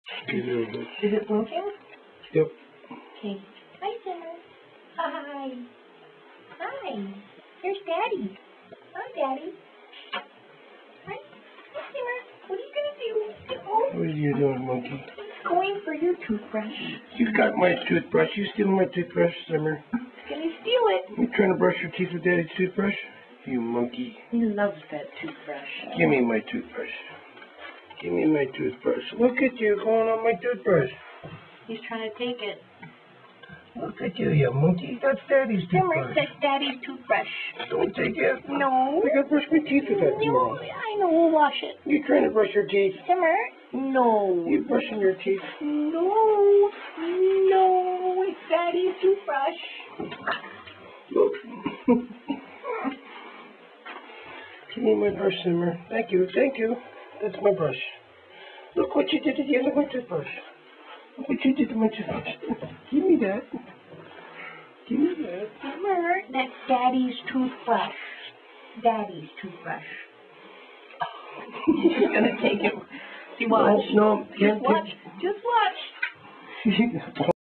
stealingtoothbrush
Ferret decides that Daddy's toothbrush with the rubber-tipped "gum-massagers" NOW BELONGS TO HIM!